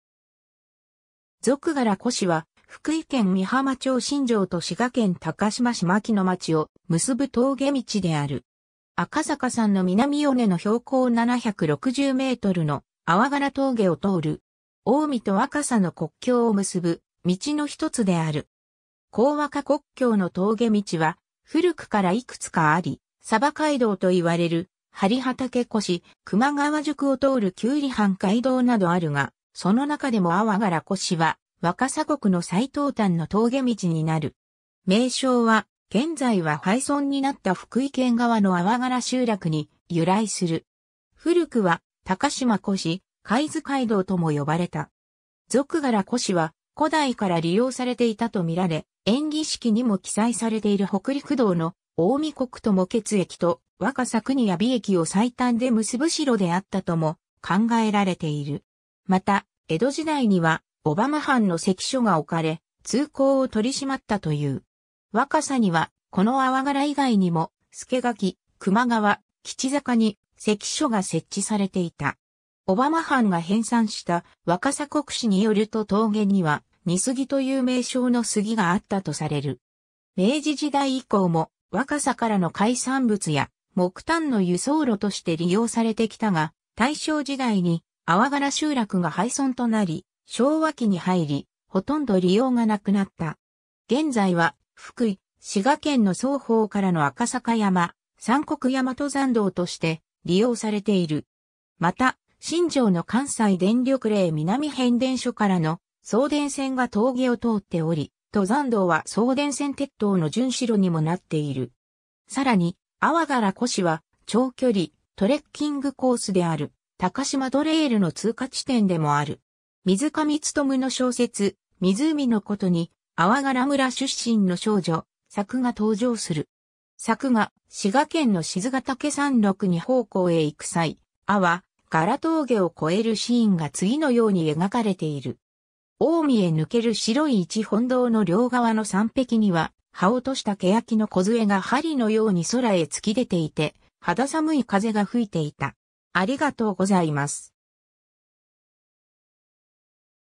粟柄越
粟柄越（あわがらごえ）は、福井県美浜町新庄と滋賀県高島市マキノ町を結ぶ峠道である。
赤坂山の南尾根の標高760mの粟柄峠を通る。
近江と若狭の国境（江若国境）を結ぶ道の一つである。
 江若国境の峠道は、古くからいくつかあり、鯖街道と言われる針畑越（根来坂）、熊川宿を通る九里半街道（現在の国道303号）などあるが、その中でも粟柄越は若狭国の最東端の峠道になる。
名称は、現在は廃村になった福井県側の粟柄集落に由来する。
古くは高島越、海津街道とも呼ばれた。
 粟柄越は、古代から利用されていたとみられ、『延喜式』にも記載されている北陸道の近江国鞆結駅と若狭国弥美駅を最短で結ぶ支路であったとも考えられている。
また江戸時代には、小浜藩の関所が置かれ、通行を取り締まったという。
若狭には、この粟柄以外にも、佐柿、熊川、吉坂に関所が設置されていた。
小浜藩が編纂した『若狭国誌』によると峠には尼杉という名称の杉があったとされる。
明治時代以降も、若狭からの海産物や木炭の輸送路として利用されてきたが、大正時代に粟柄集落が廃村となり、昭和期に入り、ほとんど利用がなくなった。
現在は、福井・滋賀県の双方からの赤坂山・三国山登山道として利用されている。
また、新庄の関西電力嶺南変電所からの送電線が峠を通っており、登山道は送電線鉄塔の巡視路にもなっている。
さらに粟柄越は、長距離トレッキングコースである高島トレイルの通過地点でもある。
 水上勉の小説『湖の琴』に粟柄村出身の少女「さく」が登場する。
さくが滋賀県の賤ヶ岳山麓に奉公へ行く際、粟柄峠を越えるシーンが次のように描かれている。
「近江へぬける白い一本道の両側の山壁には、葉を落とした欅の梢が針のように空へつき出ていて肌寒い風が吹いていた」